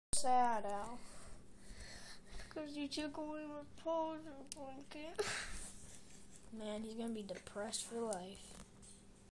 It's the sad sad sad life for him
He's so sad. I took his Paw Patrol blanket and now he's deprecated.
Sc: mrbreezystix